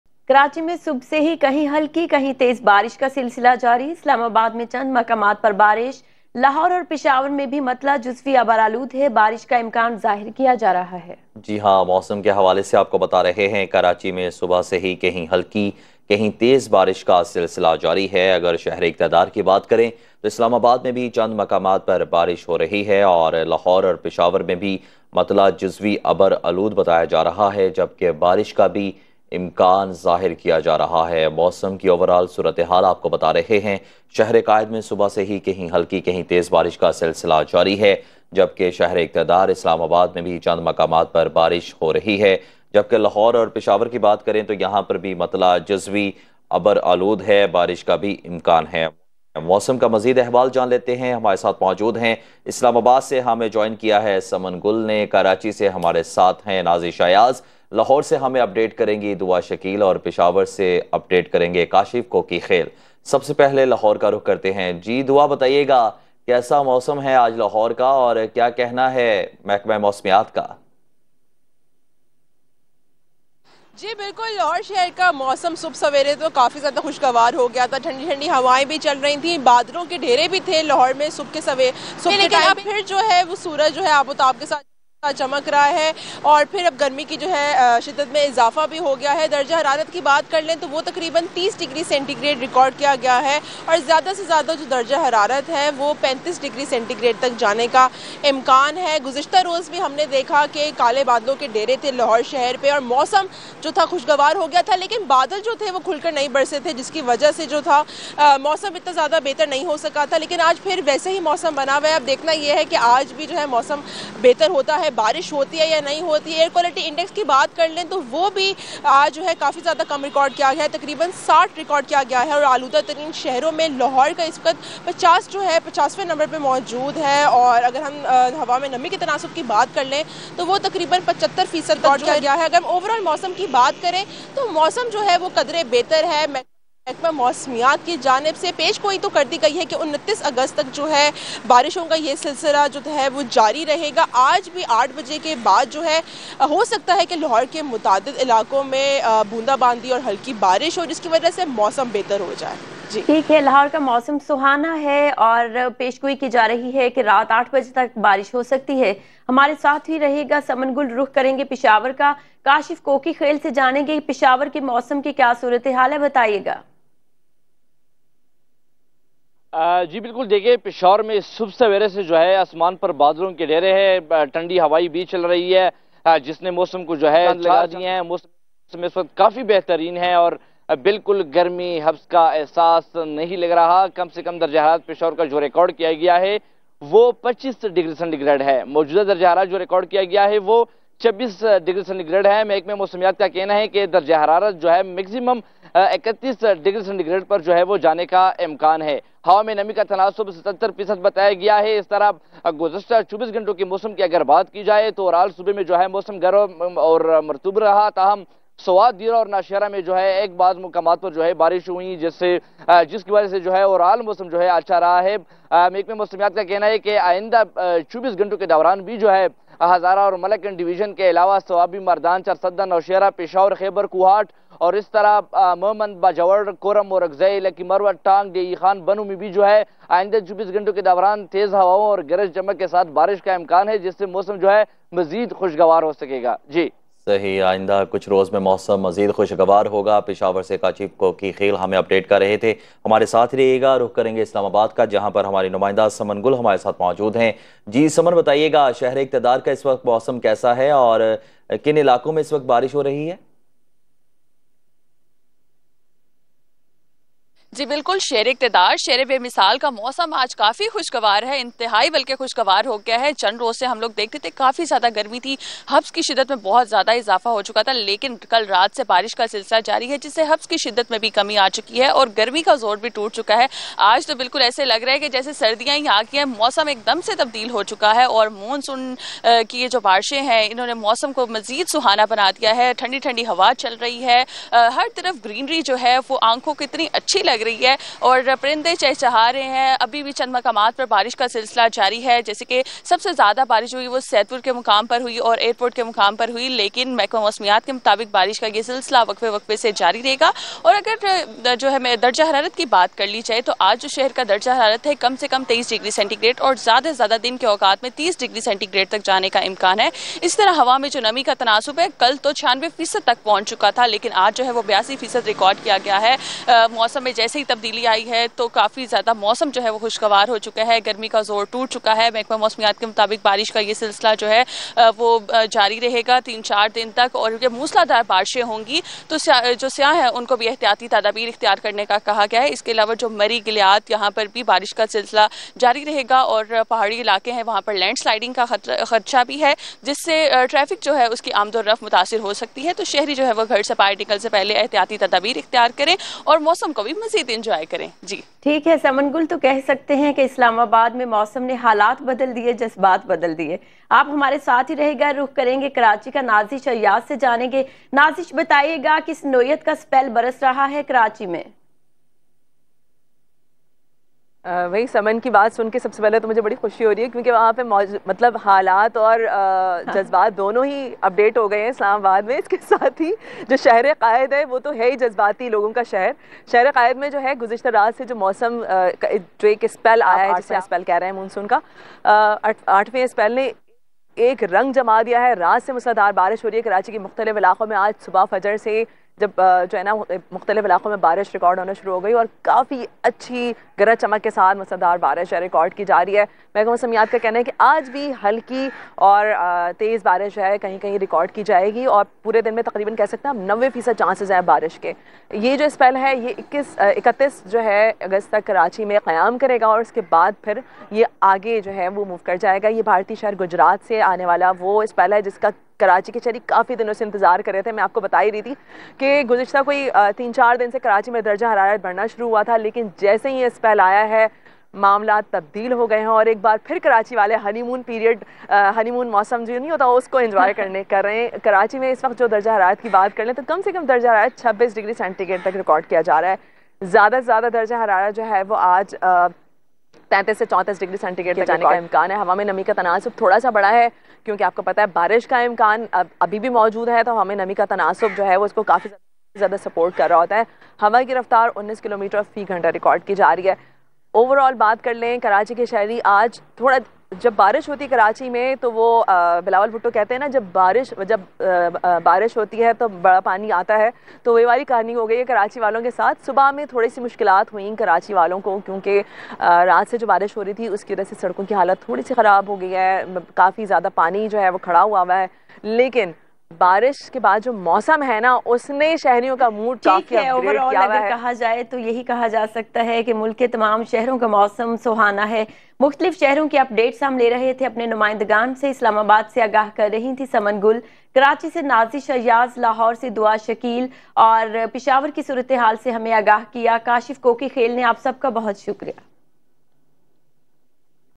Next 24 Hours is Dangerous | Terrible Rain Alert | Moon Soon Last Spell | Pakistan Weather
Pakistan mein agle 24 ghantay intehai khatarnak sabit ho sakte hain, jab ke Monsoon ka aakhri spell apni shiddat par hai. Shadeed barish aur tufan ka khatra barh gaya hai. Aap apne ilaqon mein kis tarah se mehfooz reh sakte hain? Jaanein Pakistan ke mausam ki nayi taaza tareen update is video mein. Dekhte rahiye Suno News HD!